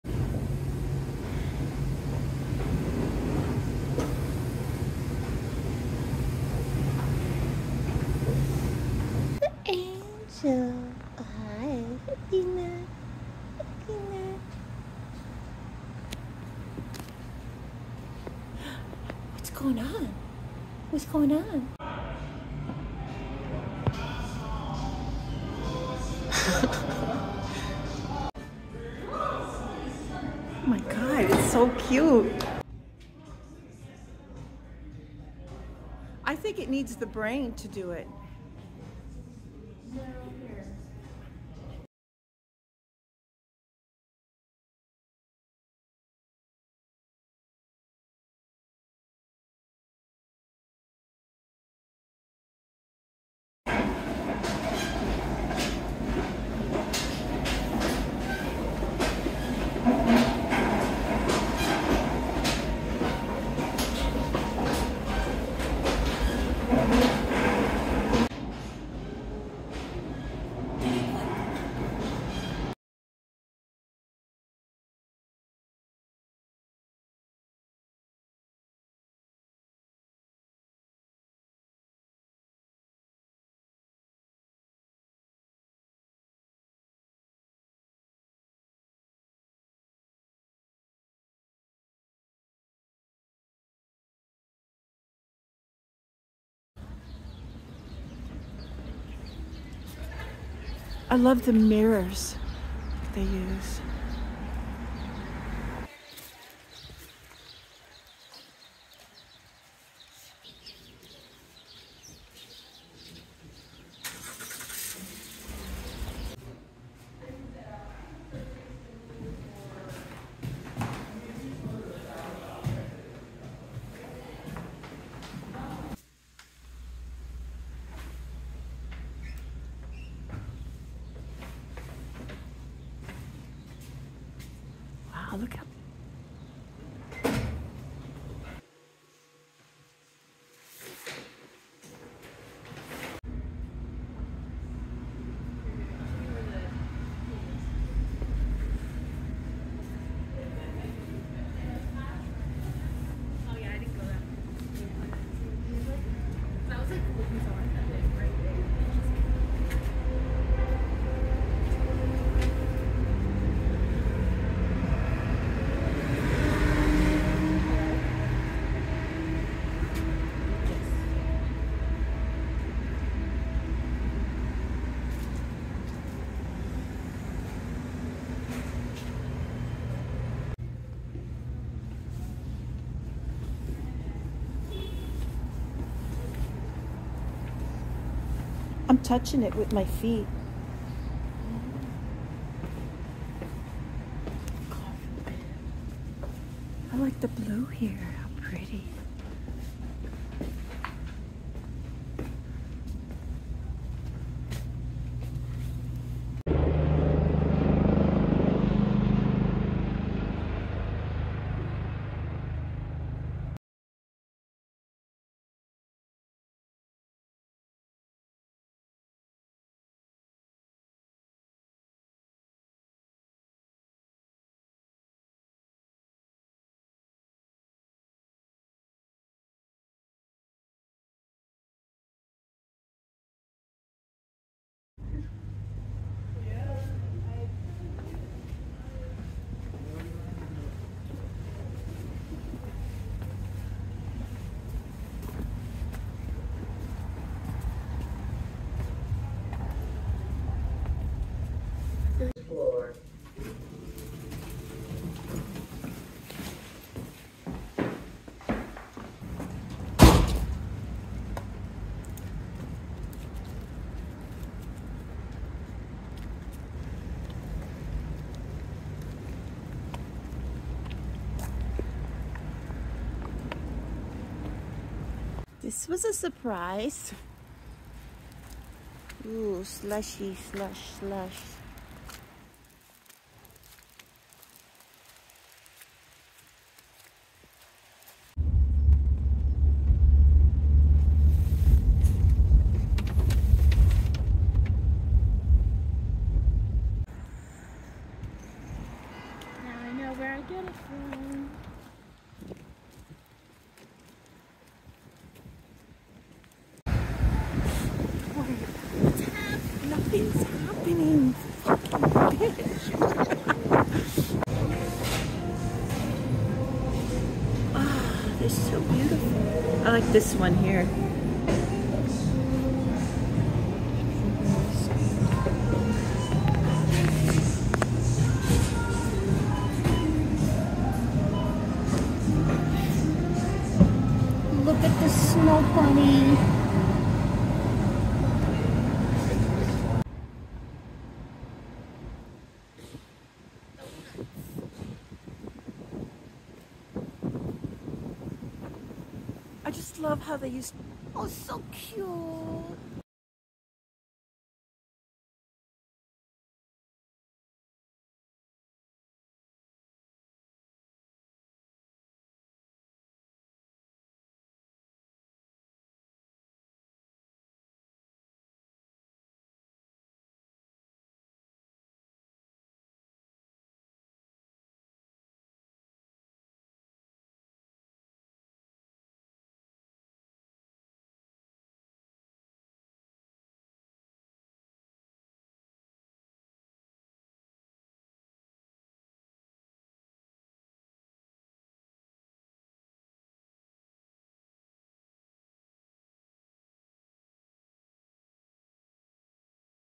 A trip to Philadelphia to see the girls 💕
video taken February 11th through 14th, 2024